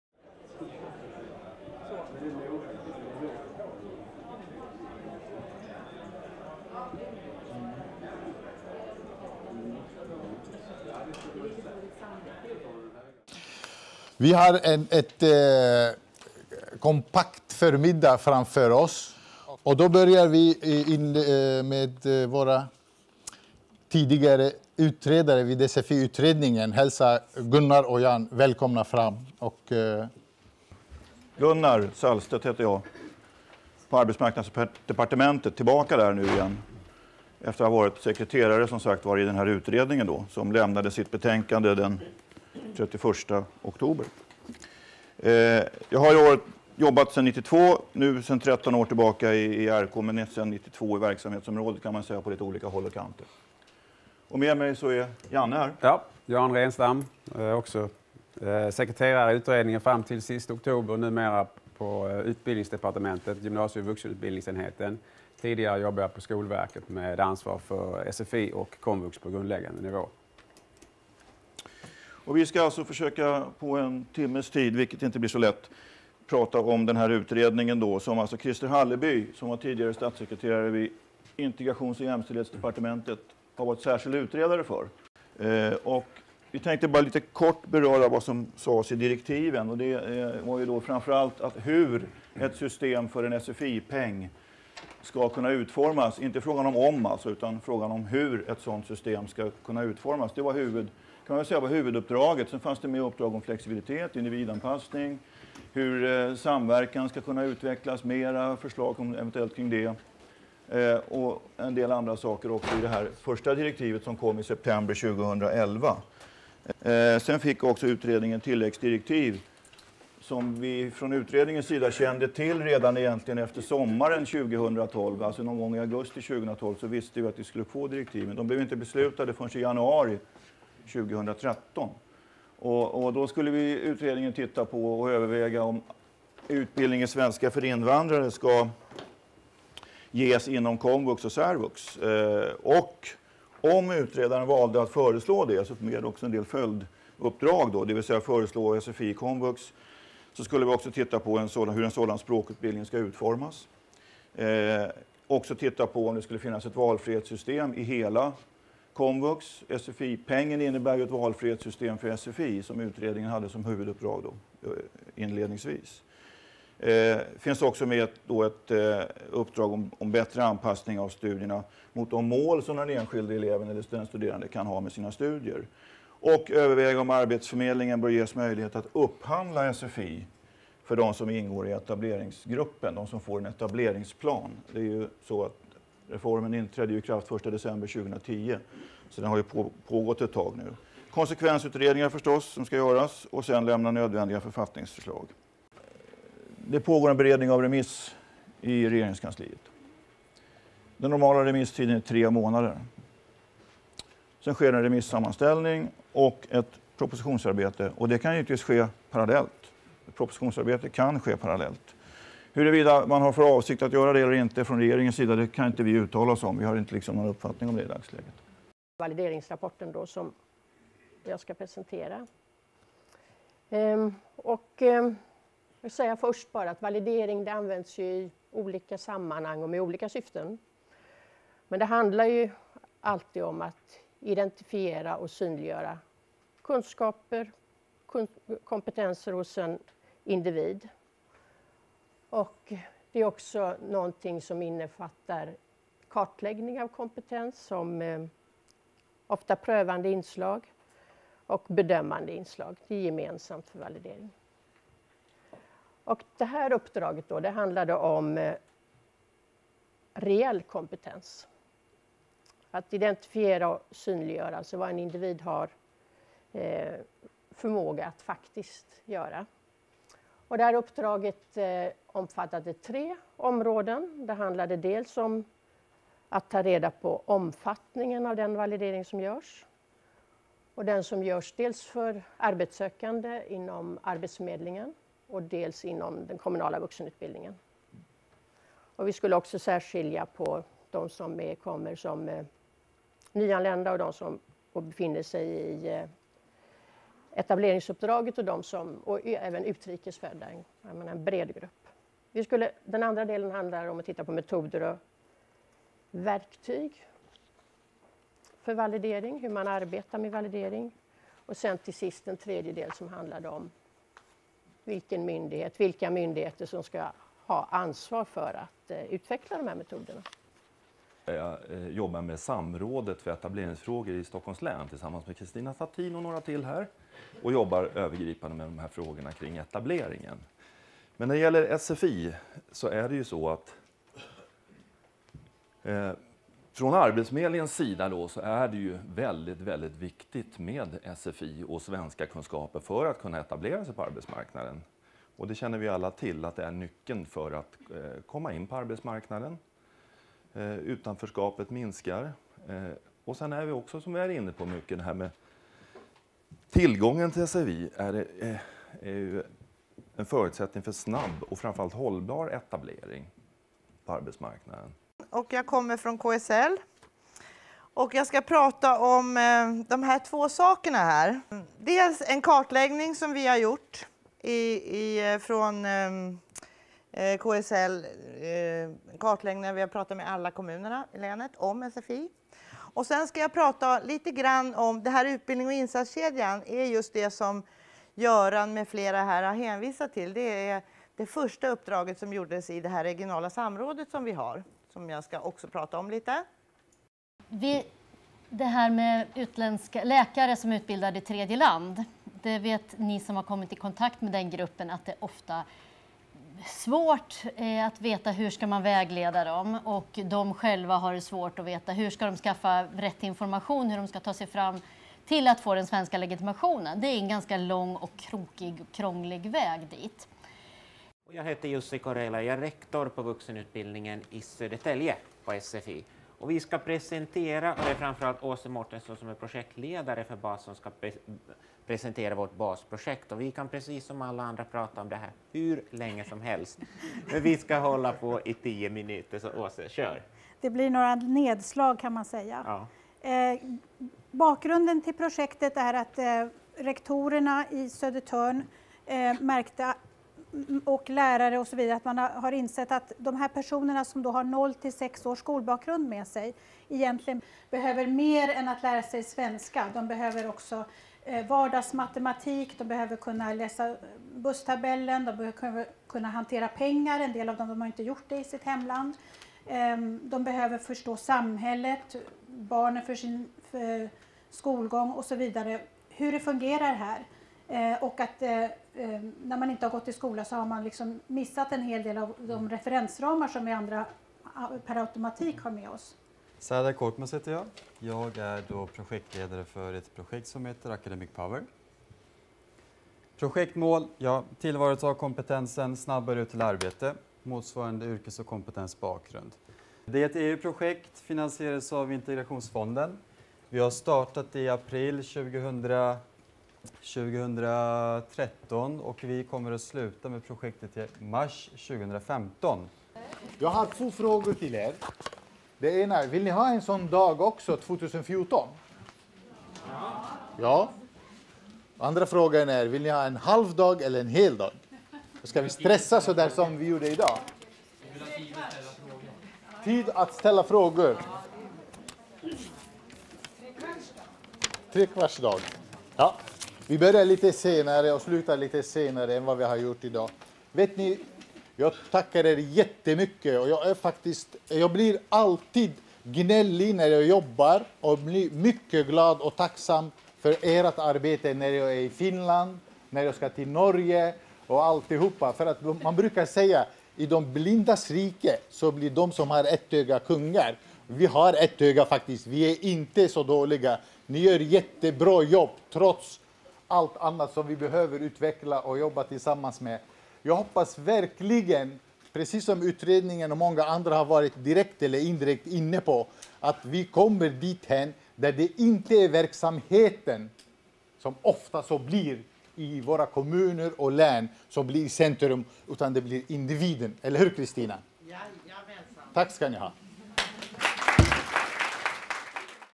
Resumé av seminarum om SFI i Stockholms län
Föredraget hölls på Länsstyrelsens seminarium om Svenska för invandrare (SFI) i Stockholms län, den 18 november 2013, där lokala, regionala och nationella aktörer möttes för kunskaps- och erfarenhetsutbyte.

Under seminariet presenterades aktuella utredningar och kartläggningar och aktuellt utvecklingsarbete av SFI inom Stockholms län.